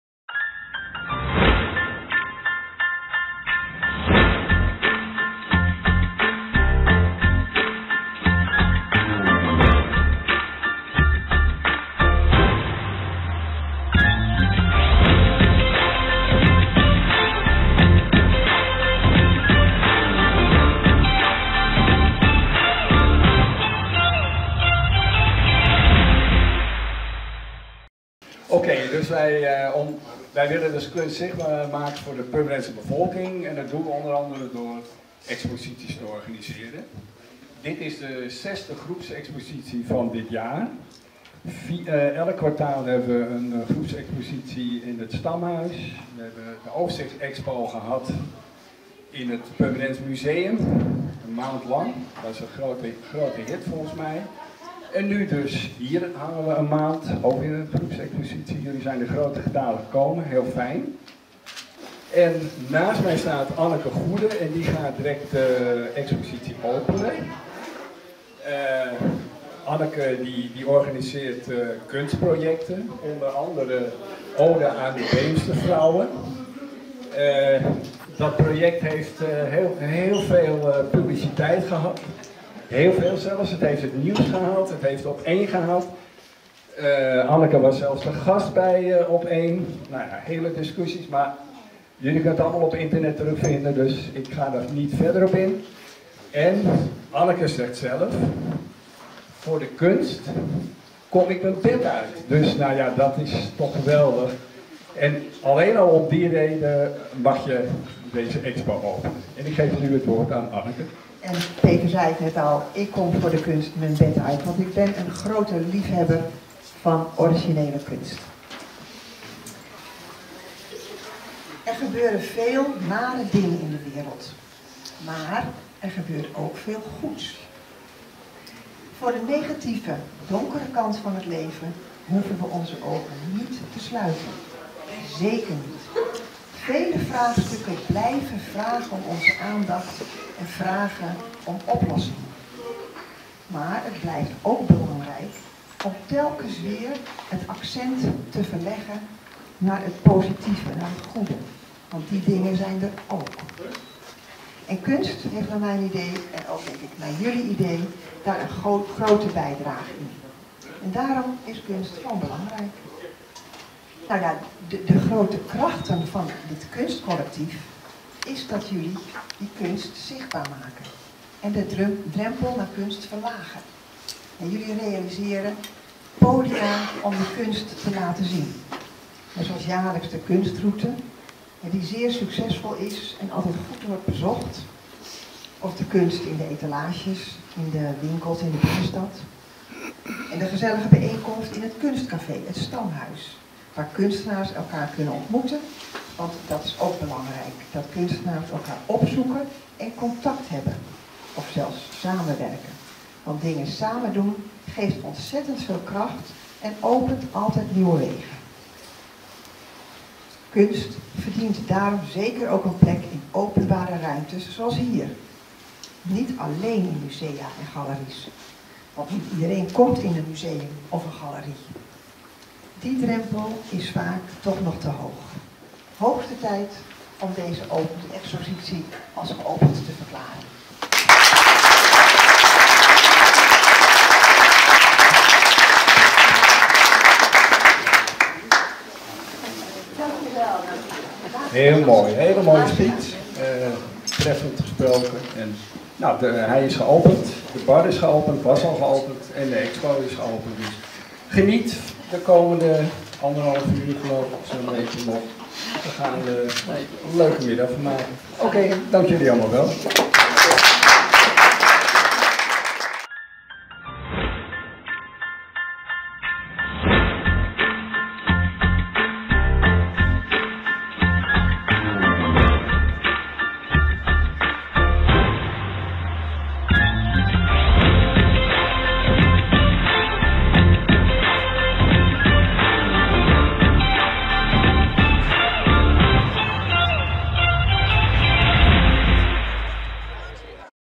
2023-12-01 - Kunst in het stadhuis van Purmerend
Op 1 december 2023 hebben diverse kunstenaars van het kunstcollectief Purmerend enige van hun kunstuitingen mogen presenteren in het gemeentehuis van Purmerend. Het doel van dit alles is om kunst in openbare ruimten voor meer mensen zichtbaar te maken i.p.v. dat kunstwerken alleen in galerieën ten toon worden gesteld.  Peter Elias als voorzitter van het kunstcollectief heeft de inleiding gedaan waarop door Anneke Goede de opening werd verricht. Alle aanwezigen konden van de aanwezige kunstobjecten genieten onder het genot van een hapje en een drankje in de kantine van het gemeentehuis.